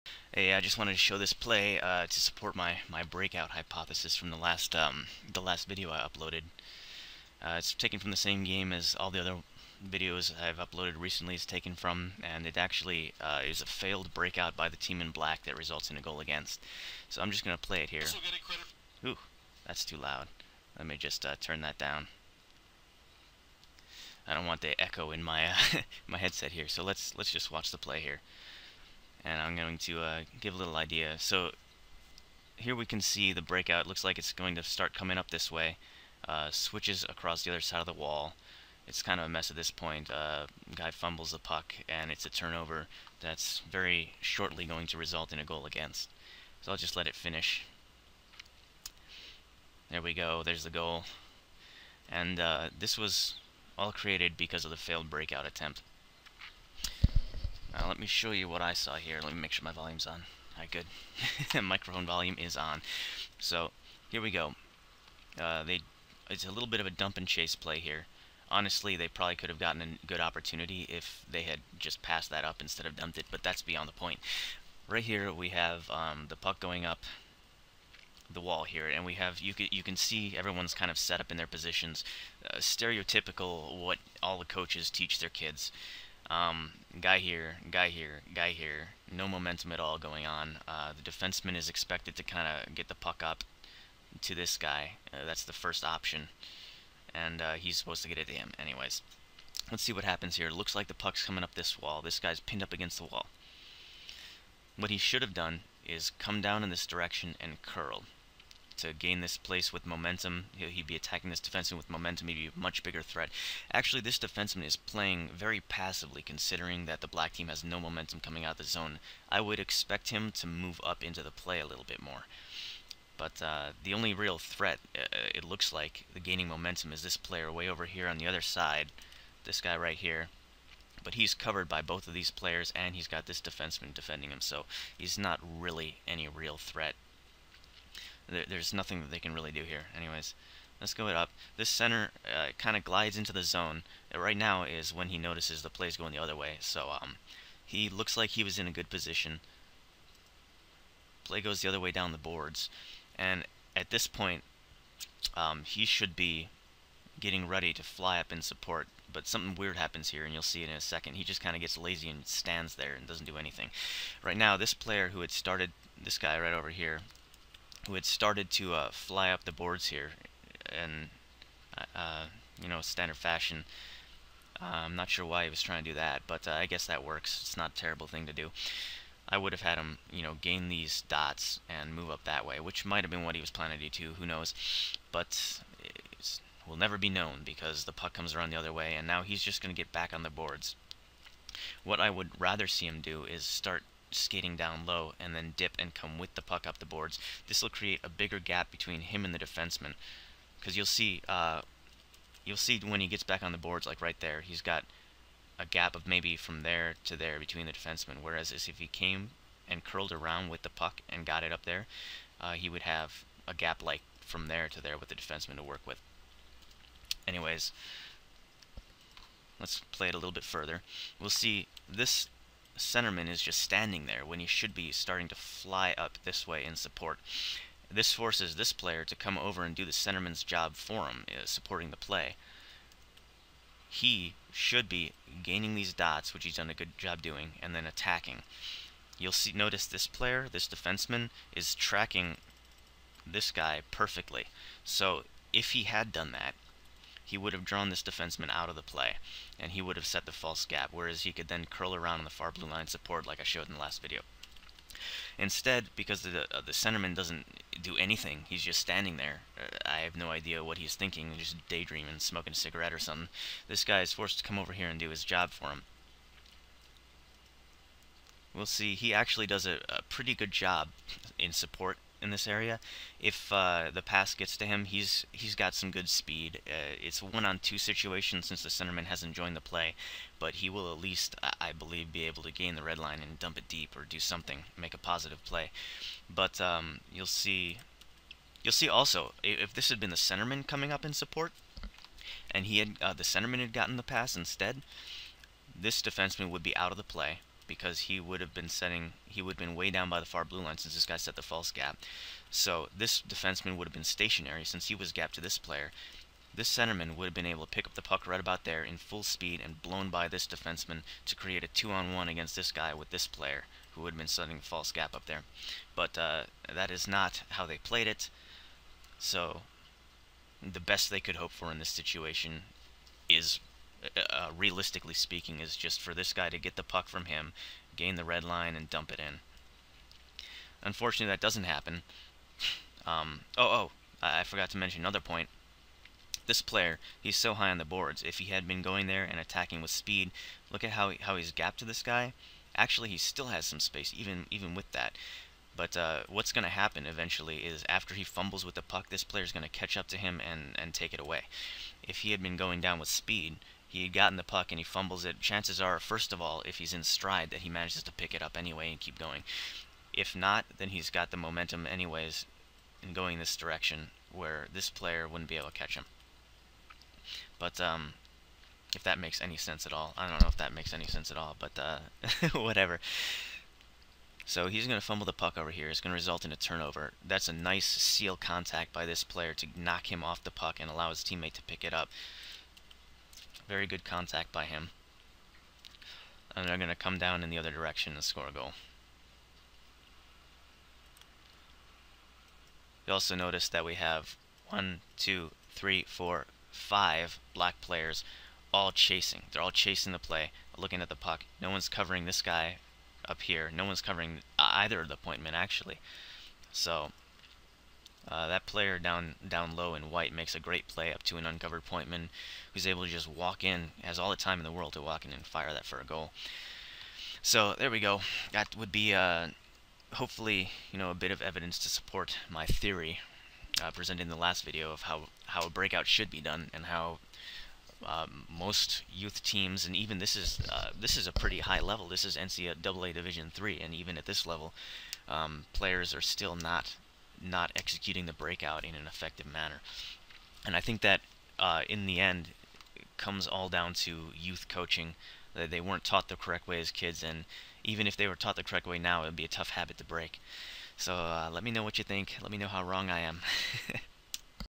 Thinking The Game: Breakout Video Example
A video example to illustrate the points I made in my last video regarding how the commonly taught breakout technique is, in my opinion, severely flawed and ineffective in today's game.